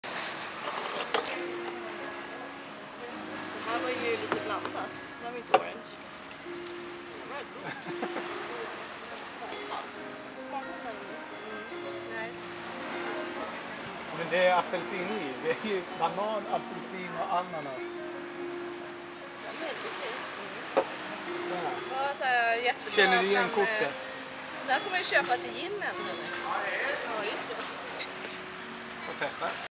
Koh Lipe 2013
Kulle testfilmar med deras systemkamera när vi var i thailand i Januari närmare bestämt på Koh Lipe, kul att han sparade den och jag såg den nu läääängtar tillbaka så det gör ont